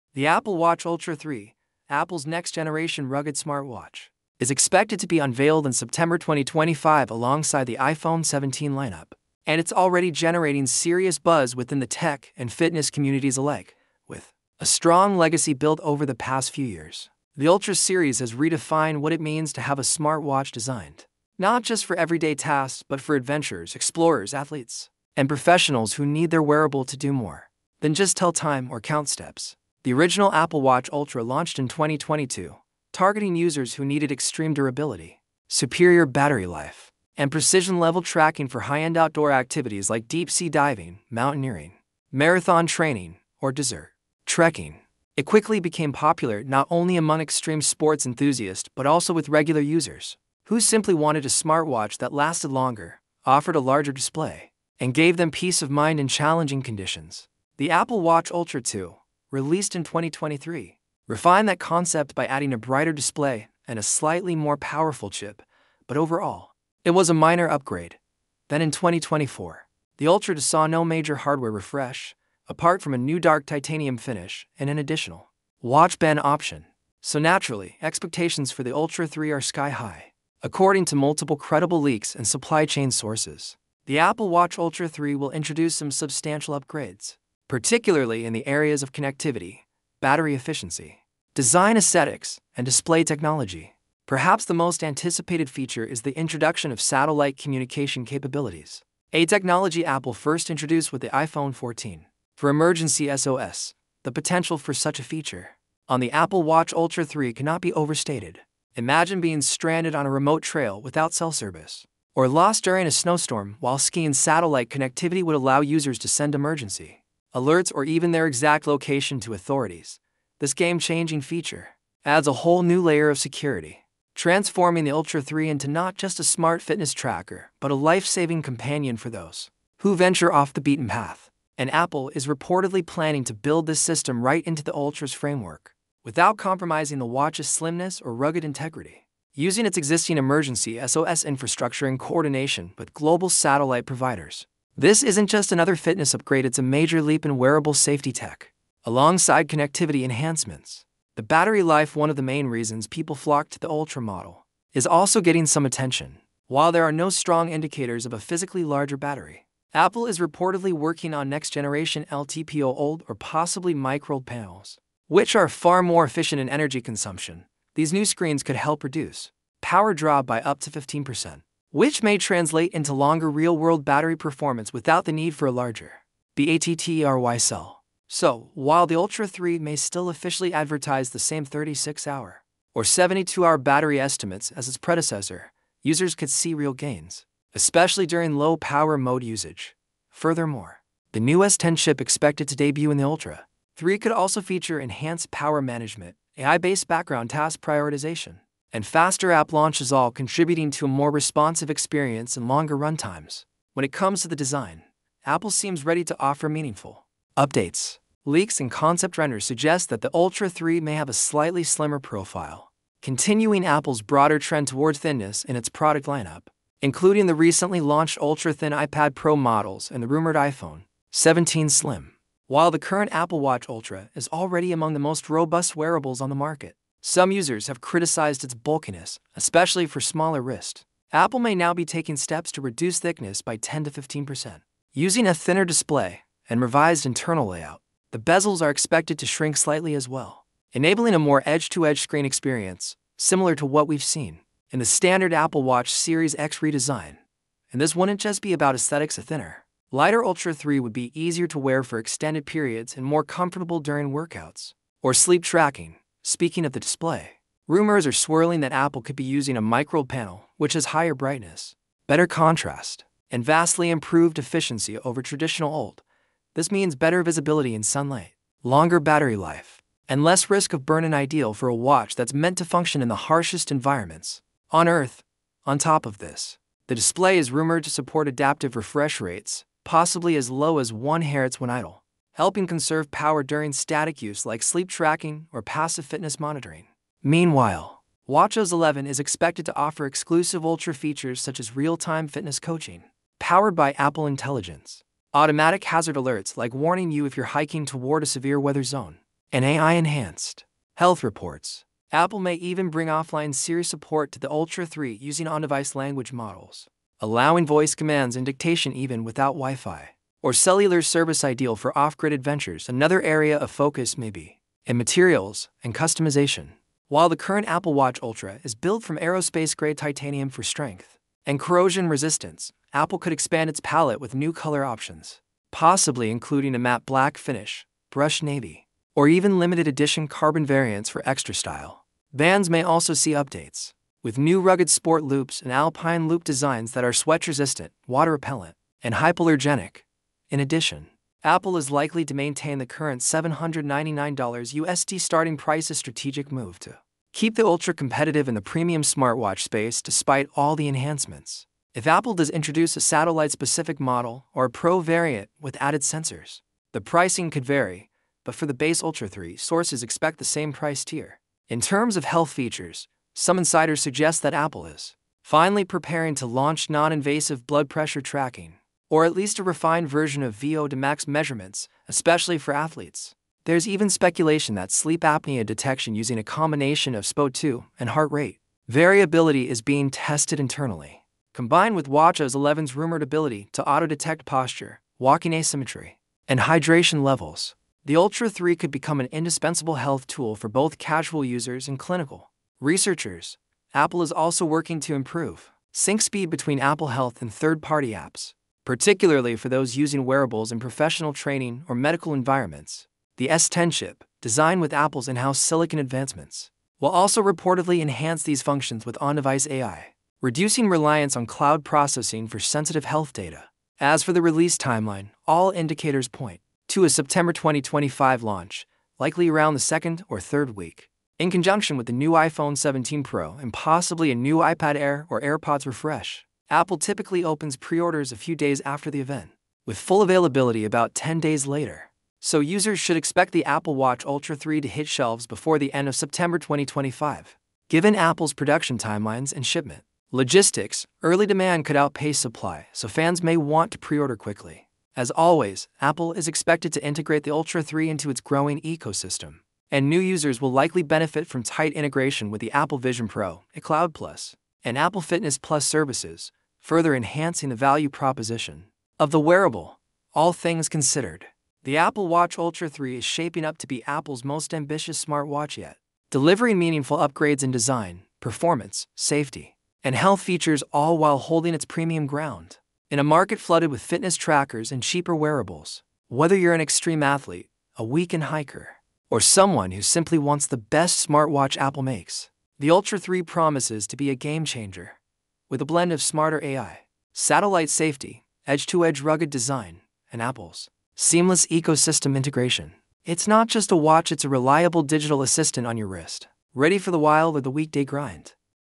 Apple Watch Ultra 3 – 2025’s Most Powerful Smartwatch is Almost Here! ⌚🔥
Get ready for the most powerful Apple Watch ever made! In this video, we break down all the latest Apple Watch Ultra 3 leaks, new features, upgraded design, and performance changes expected in 2025. From satellite connectivity to AI-powered health tracking, this might be Apple’s biggest smartwatch upgrade yet.

We’ll cover the release date, price rumors, expected specs, new health sensors, chip upgrades, and major improvements over the previous Ultra 2 and Series 11.